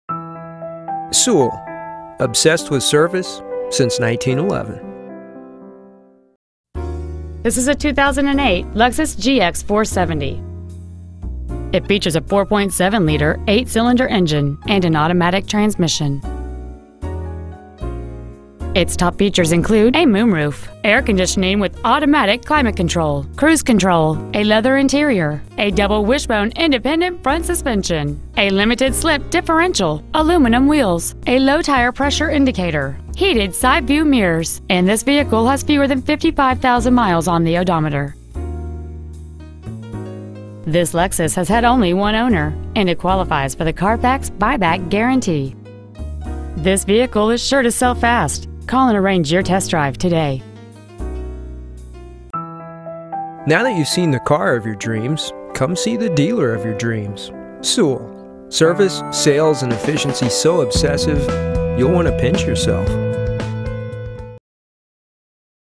2008 Lexus GX 470 Houston TX 77090
We are proud to present this 2008 Lexus GX 470.

 Year: 2008
 Make: Lexus
 Model: GX 470
 Engine: 4.7L DOHC 32-valve SFI V8 engine
 Trans.: Automatic
 Exterior: Other
 Miles: 54,346
 Interior: Other

 2008 Lexus GX 470 Houston TX